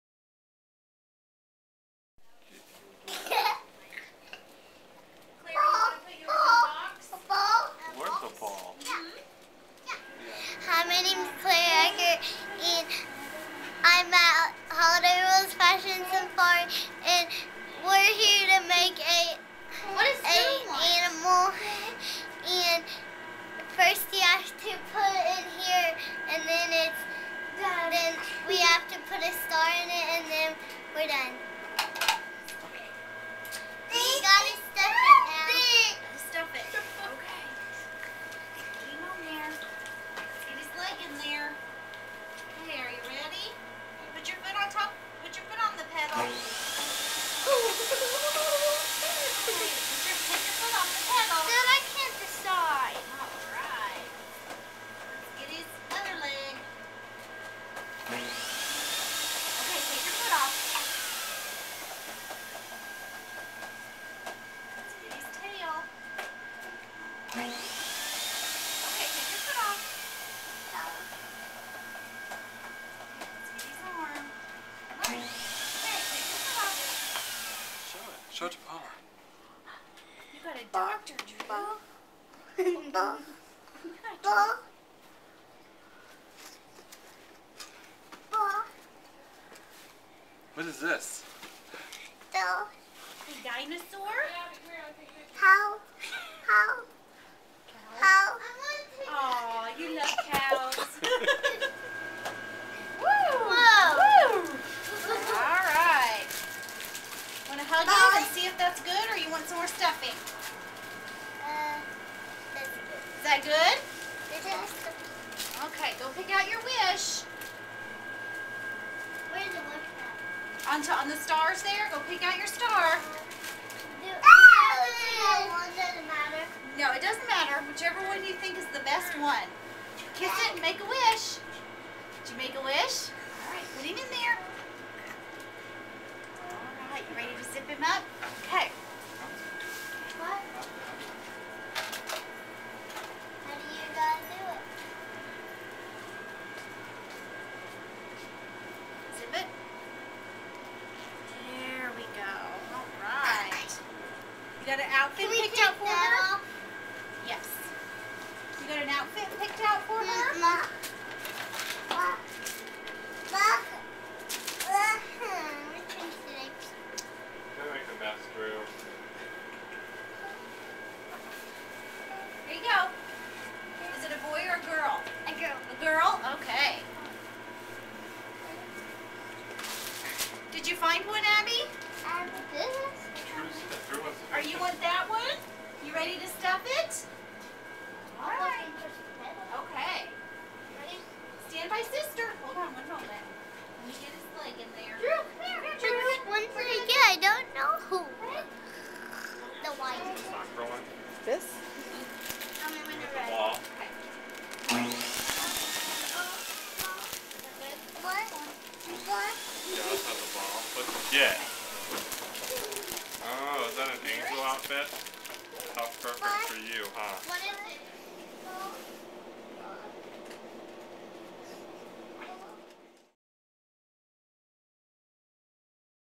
"Animaland" comes to Holiday World
Matt's kiddos visit St. Nick's Gift Shop to try out the new Animaland room at Holiday World. (Watch out for flying cows!)